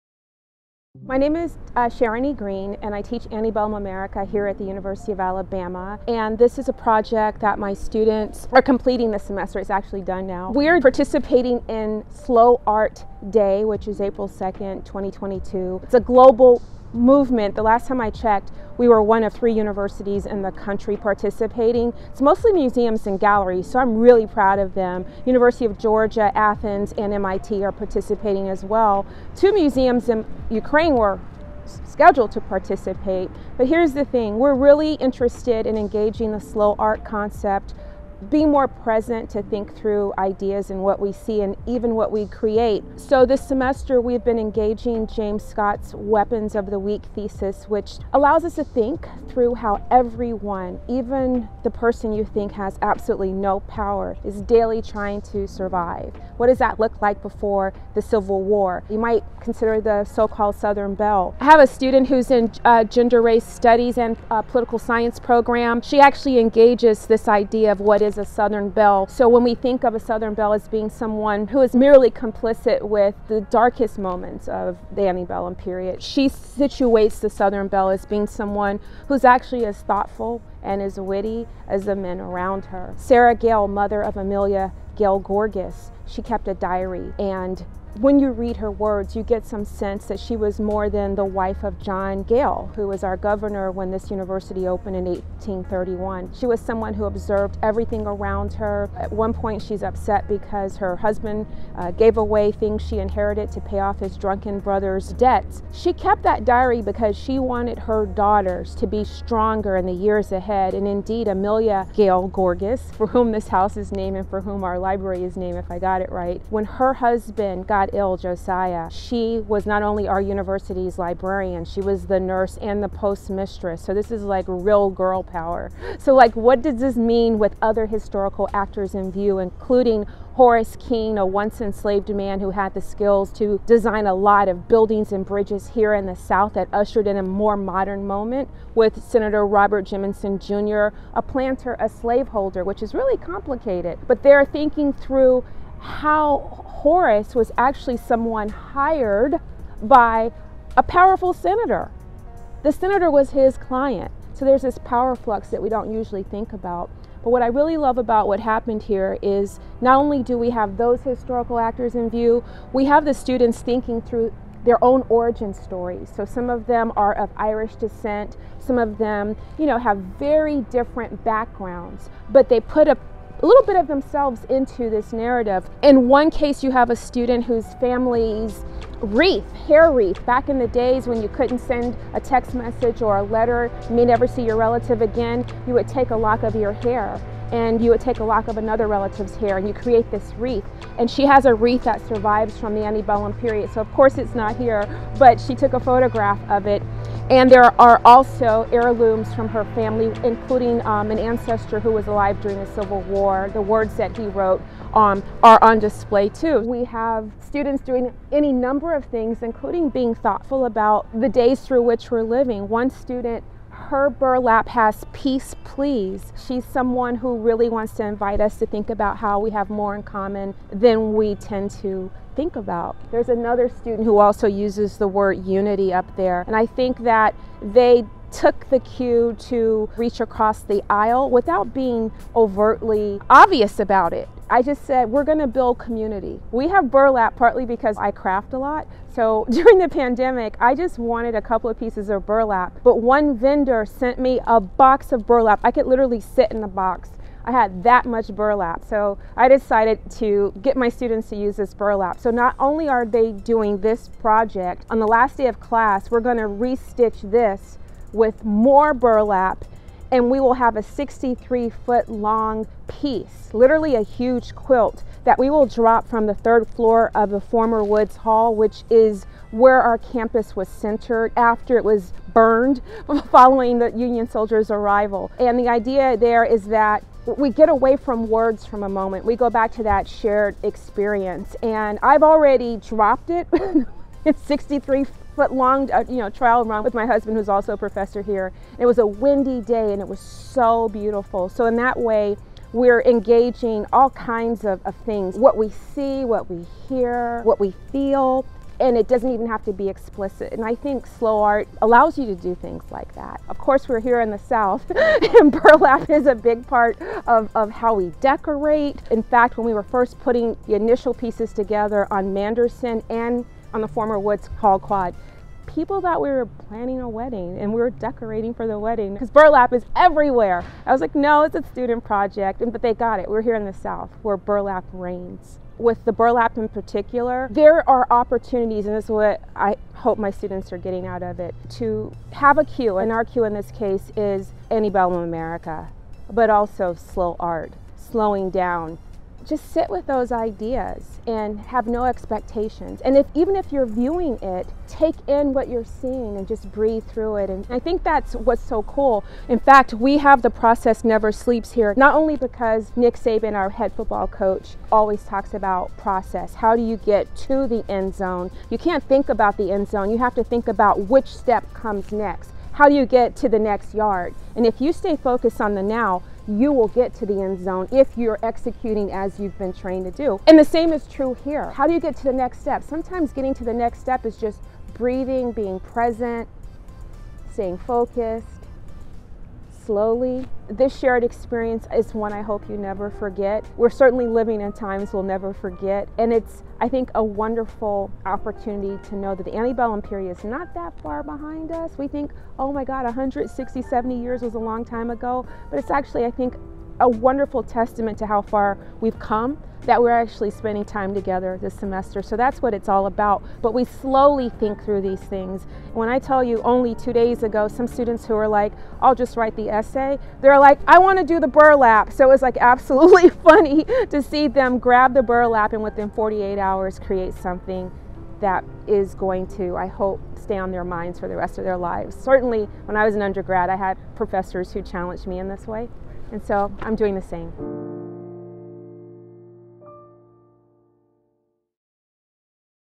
Slow Art Day 2022 at the Gorgas House Museum
This year, The Gorgas House Museum and The University of Alabama Department of History are participating in Slow Art Day, which is a global event with a simple mission: help more people discover for themselves the joy of looking at and loving art. Dr. Sharony Green guides us through the art her students created to think through Antebellum America.

• Weapons of the Weak: Everyday Forms of Peasant Resistance

00:00 - Start of Video
00:15 - What is Slow Art Day?
01:01 - Student Creations
03:56 - Why Burlap?
05:49 - The Process Never Sleeps